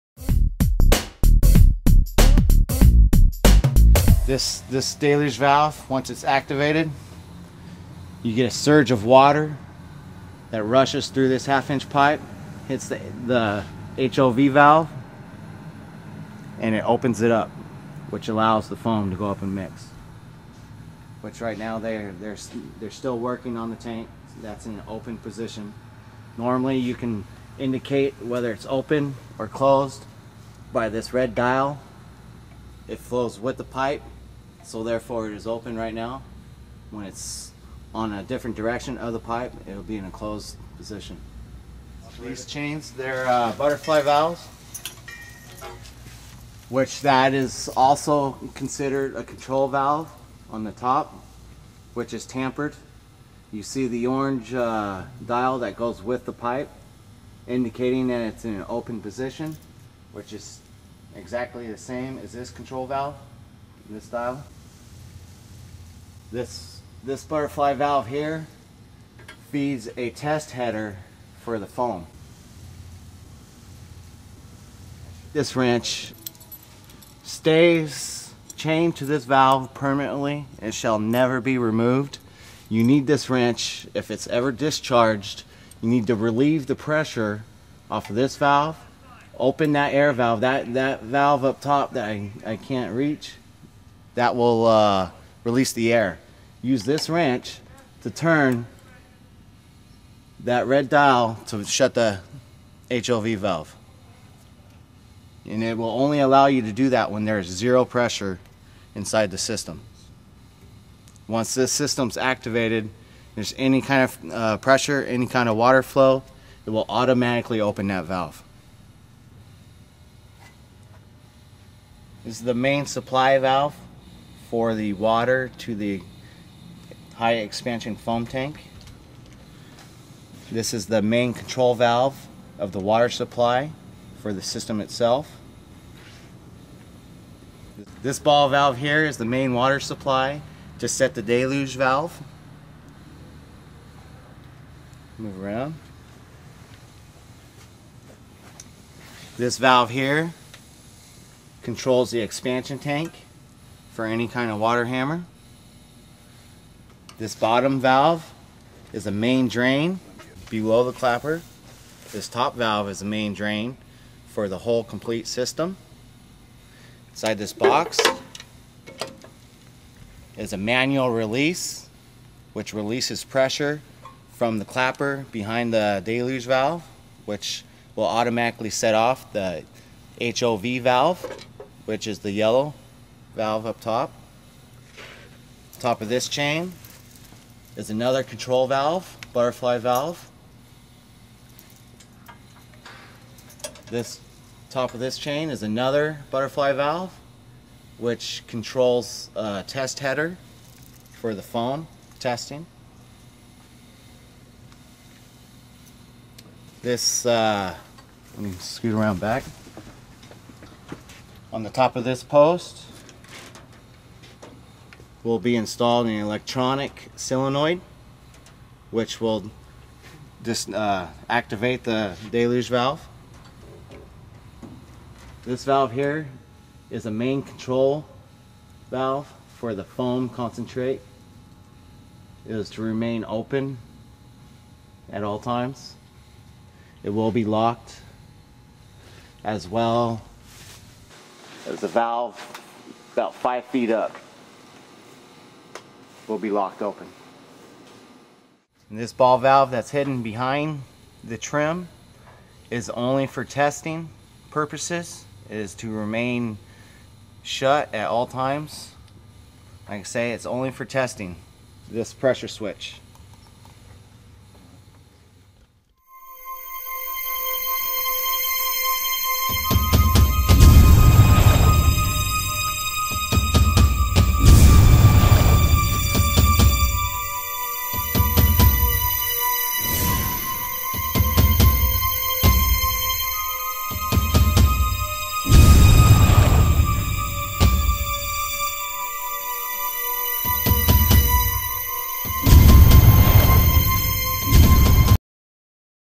Control Training - Video Services in Fairfield Ca.
Additional controls associated with the Deluge Valve.

Foam Fire Suppression System Installed by  Desert Design and Construction Corp.